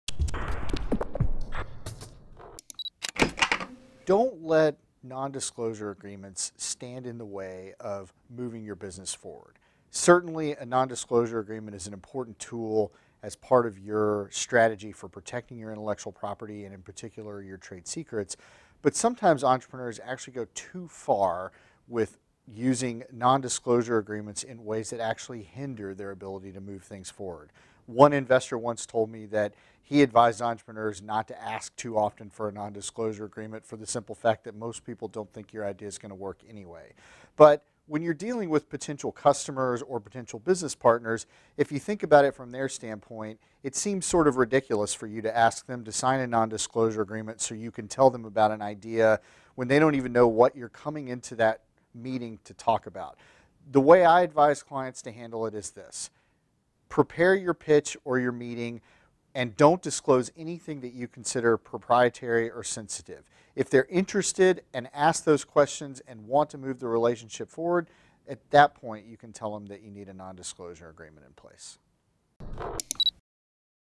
Baker Donelson's Entrepreneur Minute: Is a Non-Disclosure Agreement Always Necessary?
Chris Sloan, attorney at Baker Donelson, encourages you to not let non-disclosure agreements stand in the way of moving your business forward.